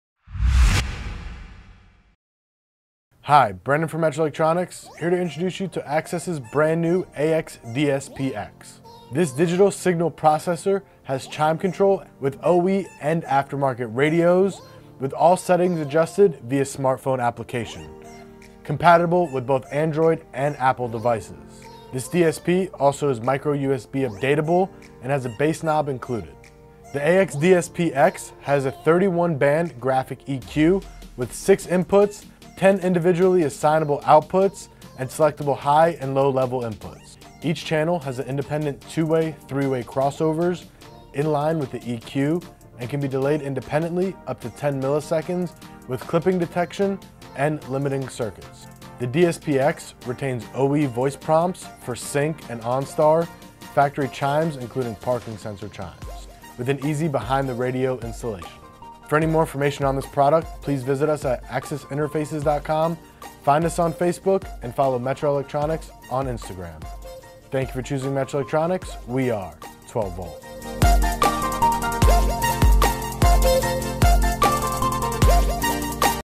Axxess AXDSP-X Digital Signal Processor with Full Tuning Control
The Axxess AXDSP-X (formerly known as the AX-DSP-X) Digital Signal Processor provides full tuning control with on-board digital amplifier turn-on capability and is a versatile and cost-effective solution for improving a sound system. Installers will have full control to fine-tune each of the 10 channel outputs individually, controlled via Bluetooth® in the free smartphone app. The app makes tuning easy with a 31 band graphic EQ and up to 10ms time delay for each independent channel, in addition to two-way/three-way crossovers in-line with the EQ. Once tuning has been completed, installers can password protect their settings to prevent anyone else from adjusting and the information is stored for future recall. Utilizing Axxess’s data interface technology, the AXDSP-X goes beyond what a traditional DSP can do and retains many of the features commonly lost when upgrading a sound system.

The AXDSP-X solution already works with a wide range of vehicle applications and systems, including OE or aftermarket radios, and fixed level audio or analog audio, in addition to non-amplified vehicles that do not have chimes. See the AXDSP-X application chart at AxxessInterfaces.com for up-to-date, vehicle specific information.